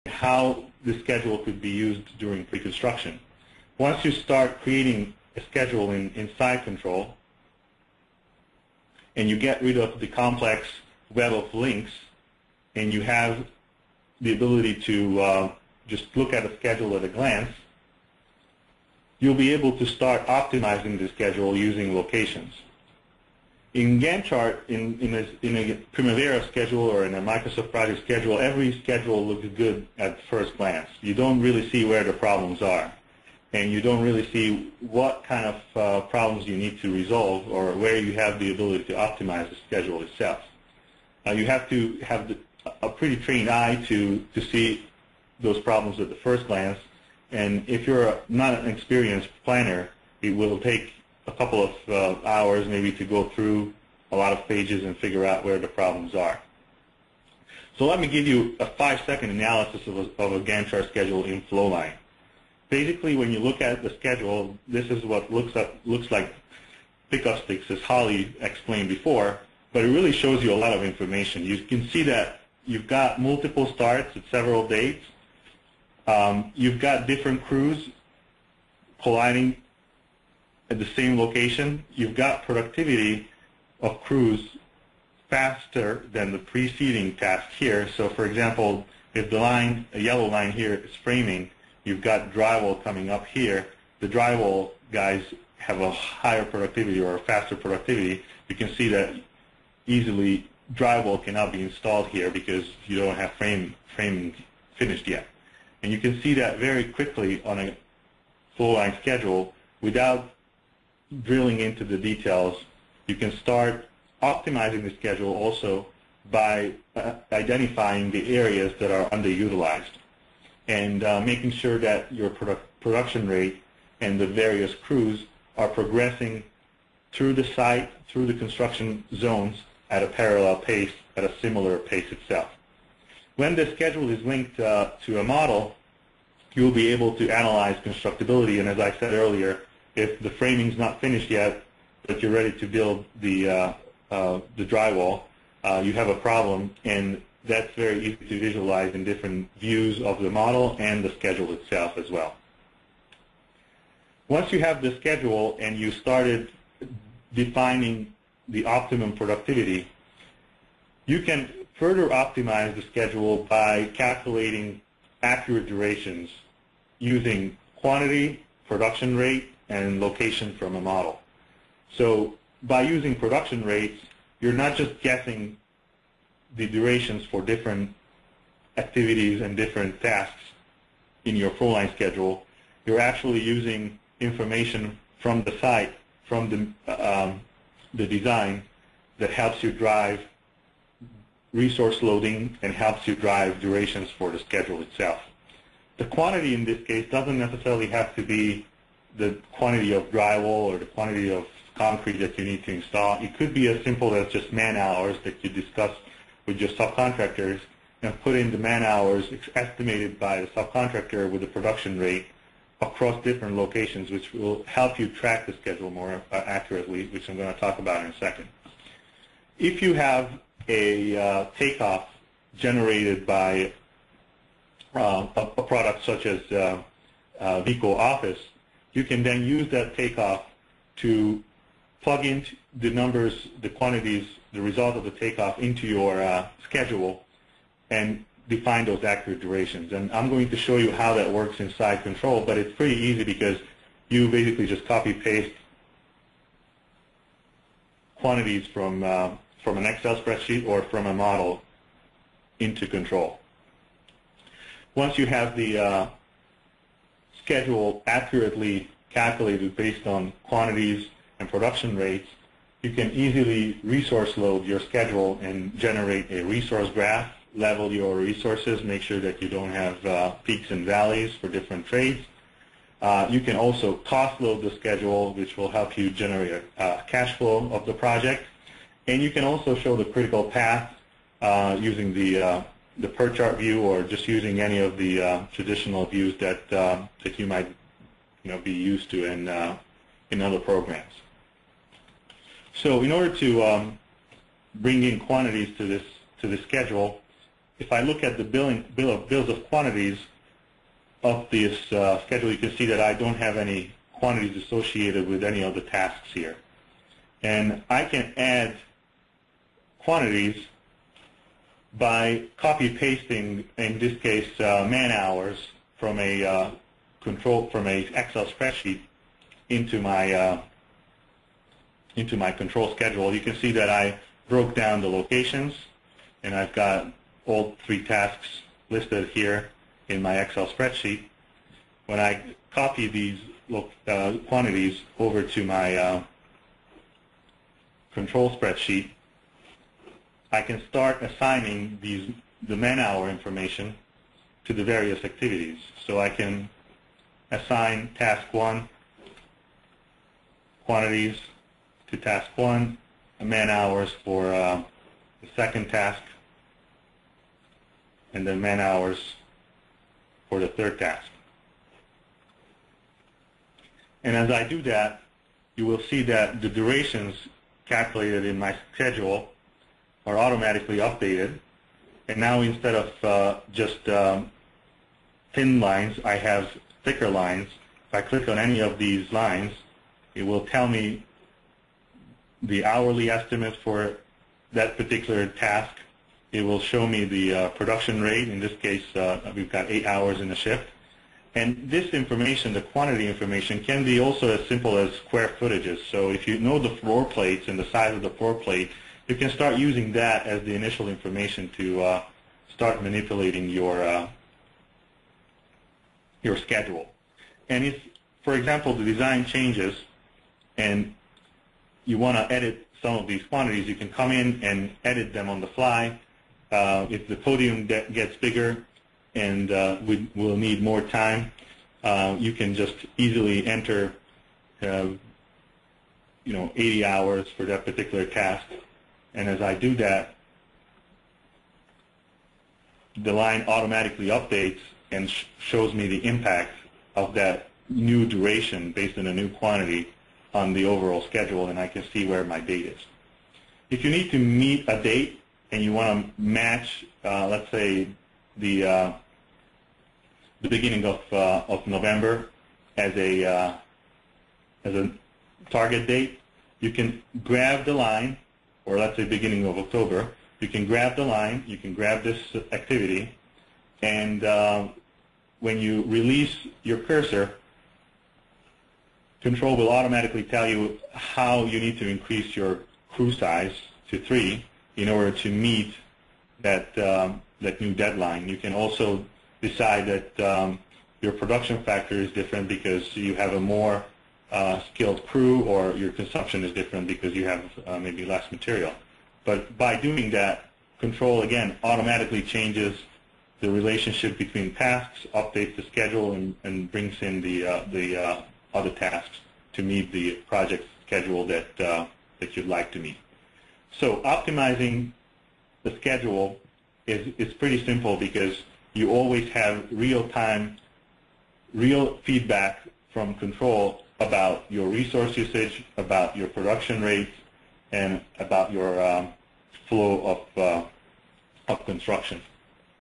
Model-Based Scheduling with Vico Control.mp4
Model-based schedules hold a unique advantage: construction-caliber quantities derived from the BIM model geometry combined with productivity rates and locations drive a much more precise schedule. Industry research shows an average of 10% schedule compression with no additional risk to the project.